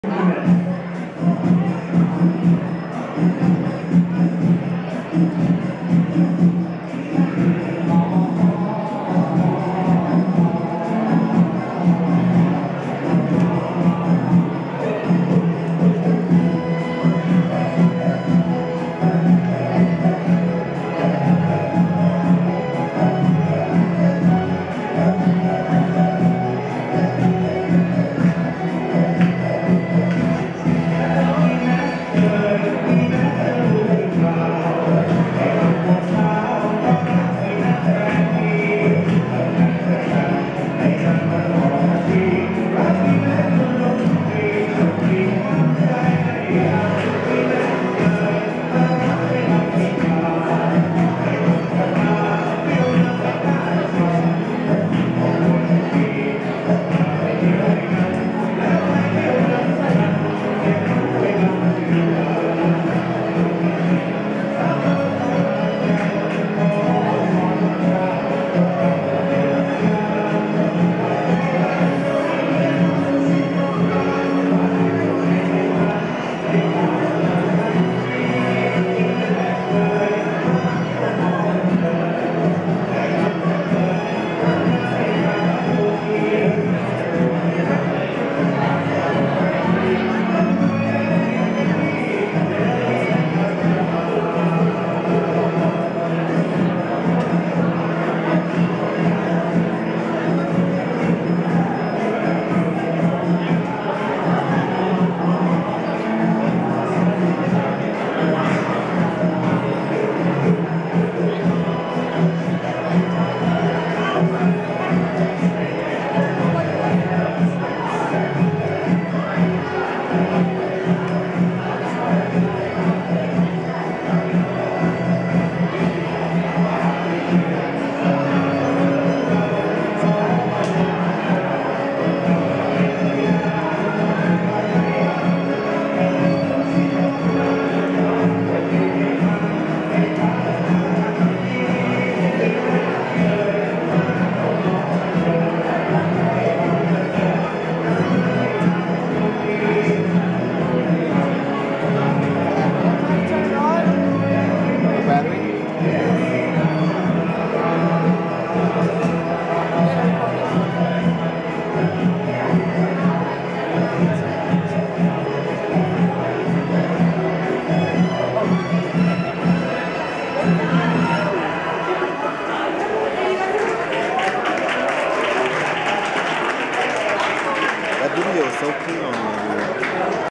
Thai Folk Song at UW Parkside Winter Cultural Festival (Dec. 1. 2010)
On December first, 2010, the minority ethnic groups or nationalities celebrated Winter Cultural Festival at the Main Place of the Unversity of Wisconsin Parkside.  A Thai folk song was perfromed and a Thai ethnic student, Phillip N. accompied the CD music with his Violin and I, a Myanmar student, accompany with a piano/keyboard.   It was great for us to be able to reveal our  cultures, tradition and folk songs to public.  Thanks for reading!